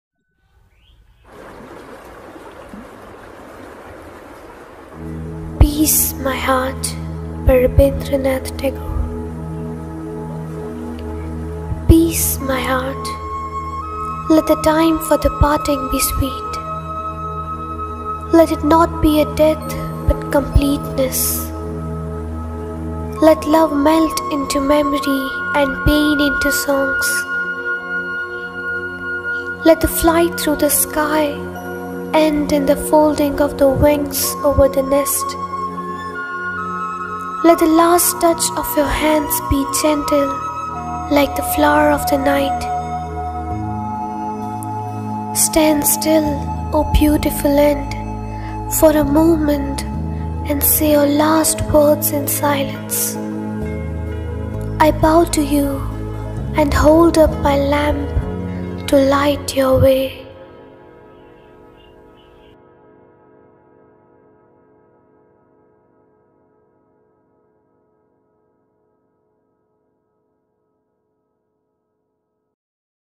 PEACE MY HEART BY RABINDRANATH TAGORE | A BEAUTIFUL DEATH POETRY | Christian Shifa
This time I'm here with something different and beautiful.
PEACE MY HEART BY RABINDRANATH TAGORE is a beautiful death poetry.
When someone we care about dies, peace may seem a long way off in the future. But it needn’t be, as this poem shows.

If we seek not to resist the passing, but to see it as a grand resolution to something beautiful – a life – we can have peace even as a loved one drifts away.

It calls us to accept that nothing is permanent and to respect that life giving way to death is the natural way of things.